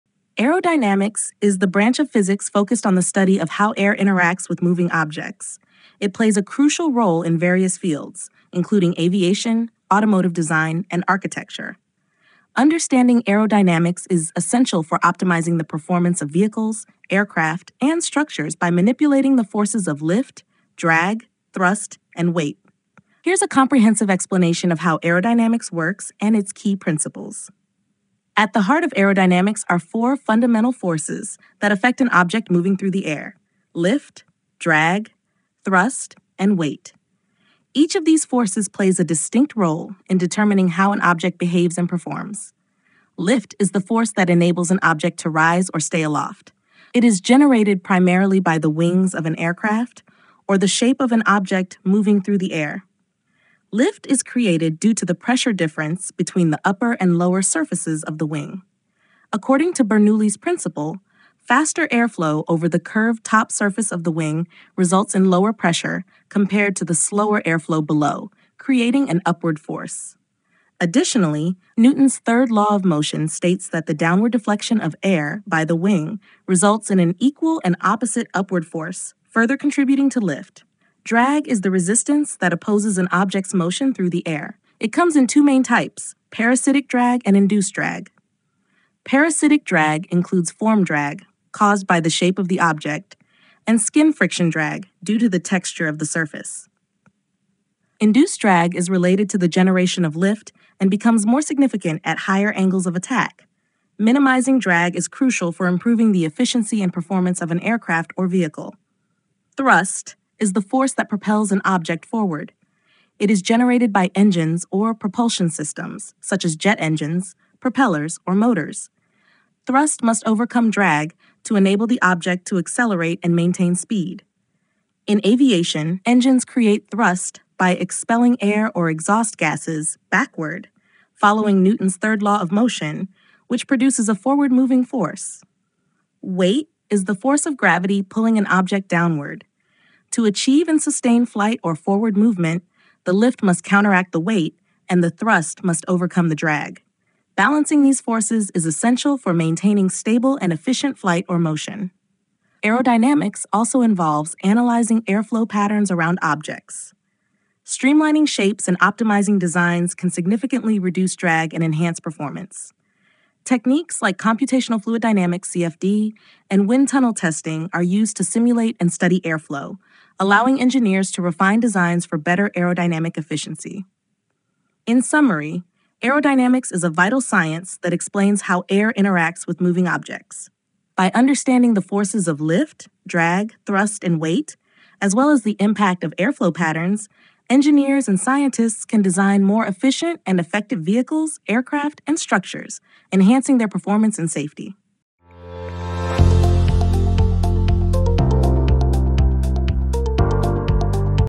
The Fundamentals of Aerodynamics How It Works and Why It Matters
**Title: The Fundamentals of Aerodynamics: How It Works and Why It Matters**

Aerodynamics is the branch of physics focused on the study of how air interacts with moving objects. It plays a crucial role in various fields, including aviation, automotive design, and architecture. Understanding aerodynamics is essential for optimizing the performance of vehicles, aircraft, and structures by manipulating the forces of lift, drag, thrust, and weight. Here’s a comprehensive explanation of how aerodynamics works and its key principles.

At the heart of aerodynamics are four fundamental forces that affect an object moving through the air: lift, drag, thrust, and weight. Each of these forces plays a distinct role in determining how an object behaves and performs.

**Lift** is the force that enables an object to rise or stay aloft. It is generated primarily by the wings of an aircraft or the shape of an object moving through the air. Lift is created due to the pressure difference between the upper and lower surfaces of the wing. According to Bernoulli's principle, faster airflow over the curved top surface of the wing results in lower pressure compared to the slower airflow below, creating an upward force. Additionally, Newton’s third law of motion states that the downward deflection of air by the wing results in an equal and opposite upward force, further contributing to lift.

**Drag** is the resistance that opposes an object's motion through the air. It comes in two main types: parasitic drag and induced drag. Parasitic drag includes form drag, caused by the shape of the object, and skin friction drag, due to the texture of the surface. Induced drag is related to the generation of lift and becomes more significant at higher angles of attack. Minimizing drag is crucial for improving the efficiency and performance of an aircraft or vehicle.

**Thrust** is the force that propels an object forward. It is generated by engines or propulsion systems, such as jet engines, propellers, or motors. Thrust must overcome drag to enable the object to accelerate and maintain speed. In aviation, engines create thrust by expelling air or exhaust gases backward, following Newton’s third law of motion, which produces a forward-moving force.

**Weight** is the force of gravity pulling an object downward. To achieve and sustain flight or forward movement, the lift must counteract the weight, and the thrust must overcome the drag. Balancing these forces is essential for maintaining stable and efficient flight or motion.

Aerodynamics also involves analyzing airflow patterns around objects. Streamlining shapes and optimizing designs can significantly reduce drag and enhance performance. Techniques like computational fluid dynamics (CFD) and wind tunnel testing are used to simulate and study airflow, allowing engineers to refine designs for better aerodynamic efficiency.

In summary, aerodynamics is a vital science that explains how air interacts with moving objects. By understanding the forces of lift, drag, thrust, and weight, as well as the impact of airflow patterns, engineers and scientists can design more efficient and effective vehicles, aircraft, and structures, enhancing their performance and safety.